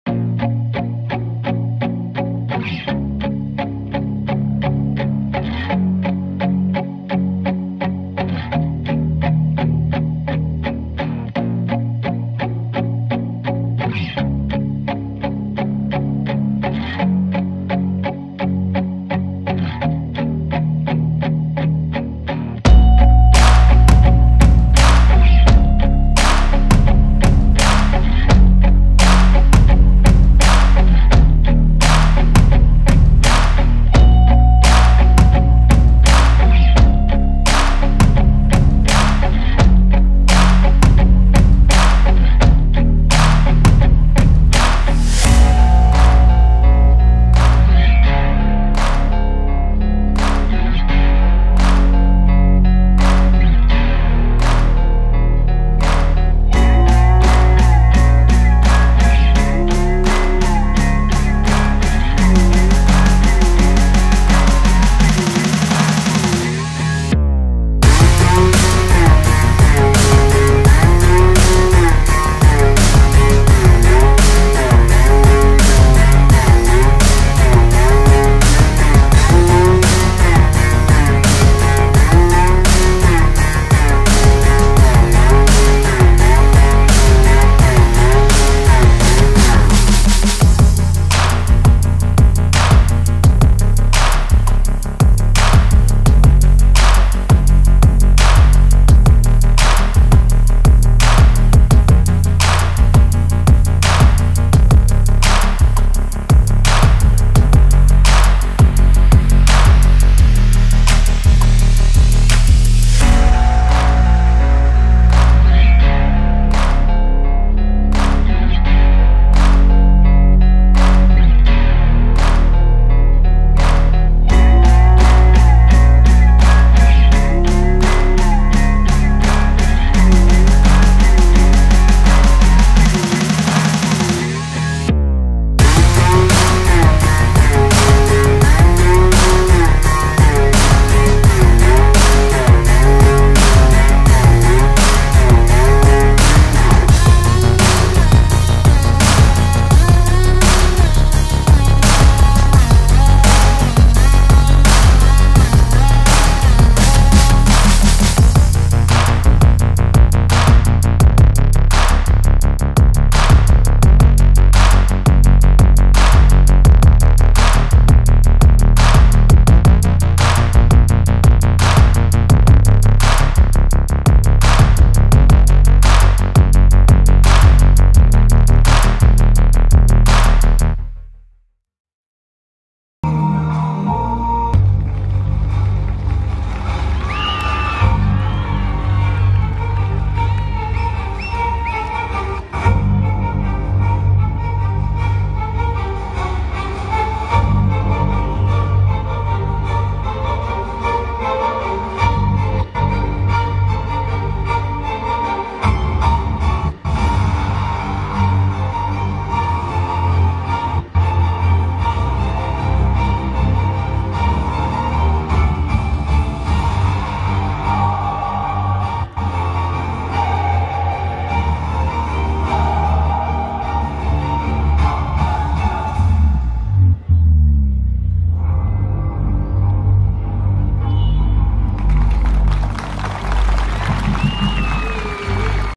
Football Skill By Alsu Daimari // National Football Championship 2022 at Dimakuchi Stadium
Hero Junior ( U-17) Women's National Football Championship 2022 at Dimakuchi Stadium